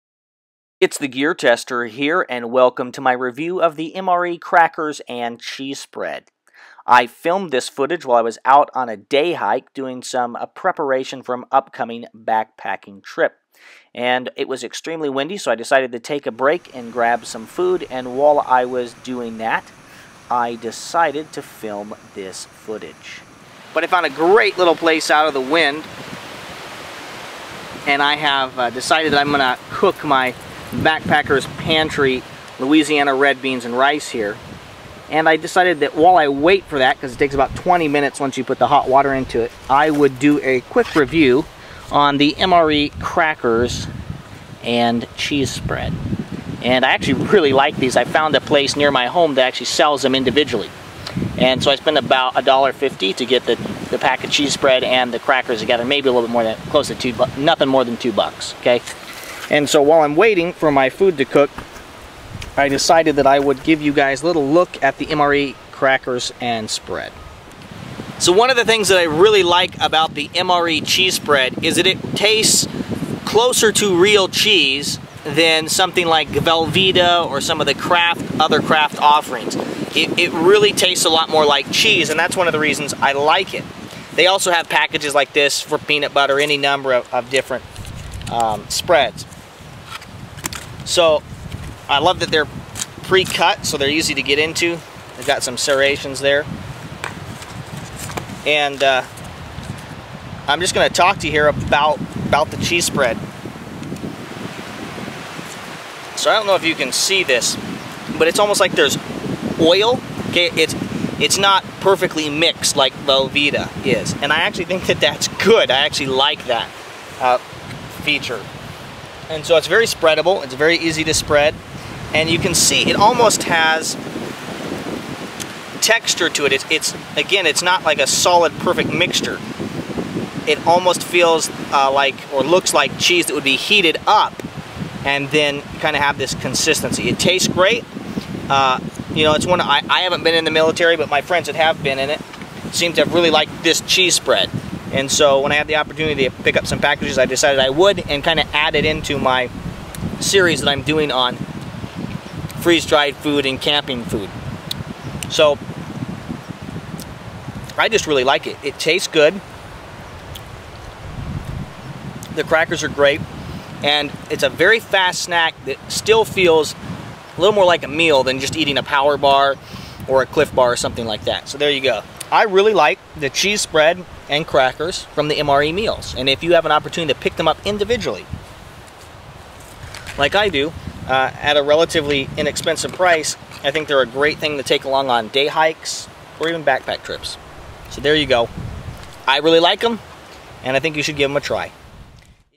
MRE Crackers and Cheese: Full Review by TheGearTester
I really like the US Military MRE's, (Meal, Ready-to-Eat). One of my favorite things about these meals is the extra snacks and goodies which accompany each of them. MRE crackers and cheese spread are two of my favorite components.